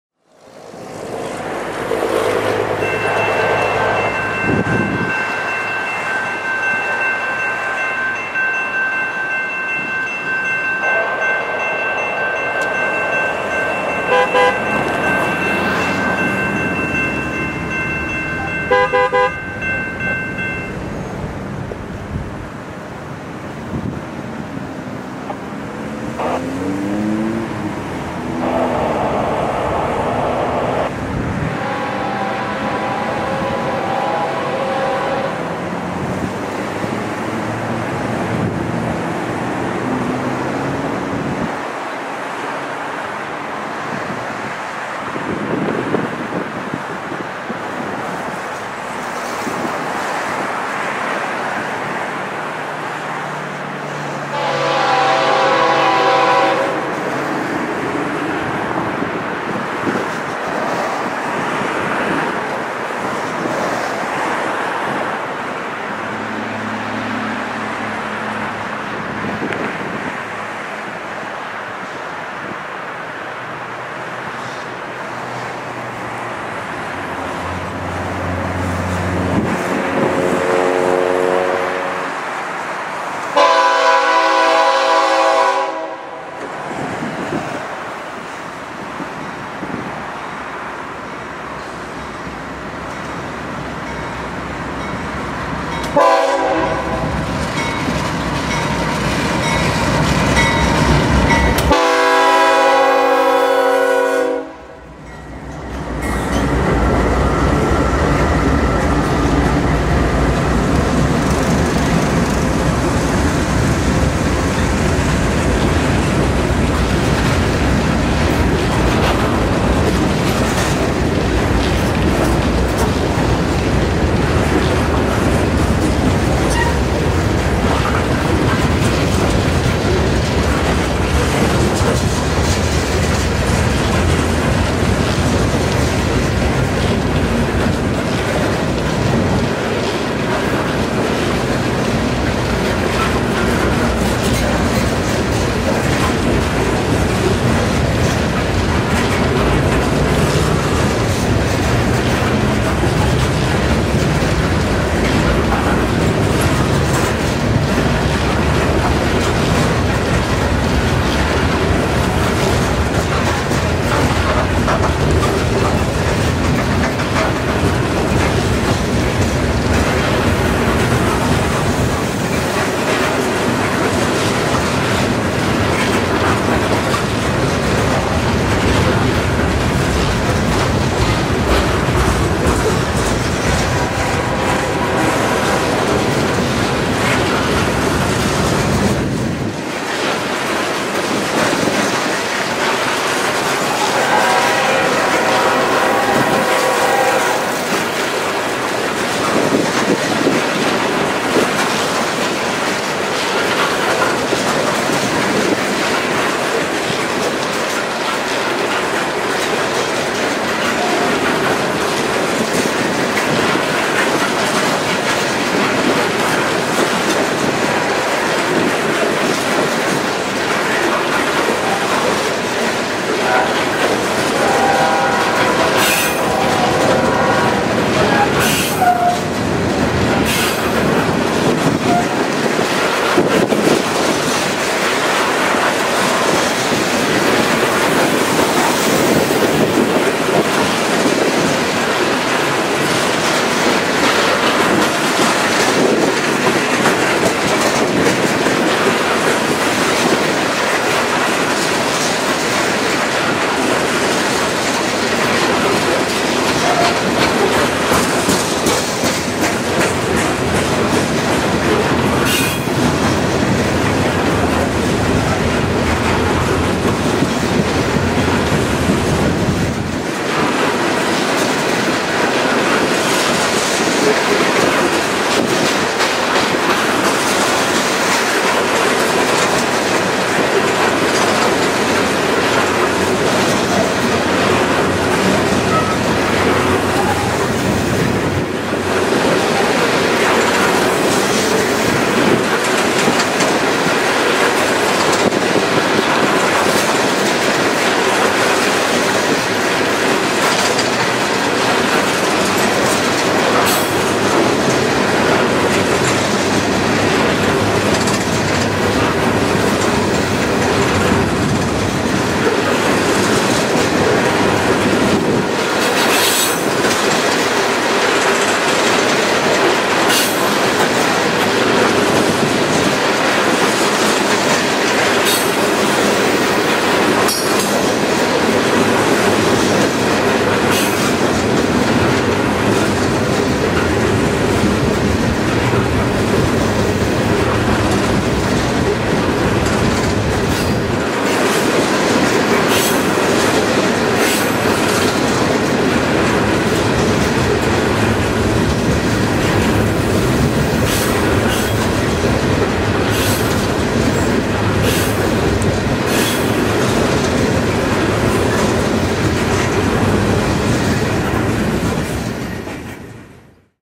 JAX 7-20-12 Orlando Coal N152
CSX N152 heads south down the A Line just south of Jacksonville with loaded coal for Orlando.

Filmed on 7-20-12 on St. Johns Avenue in Jacksonville, FL

CSX N152-15

Lynch, KY - Taft, FL

CSXT #457 AC4400CW with K5HLL Horn
CSXT #491 AC4400CW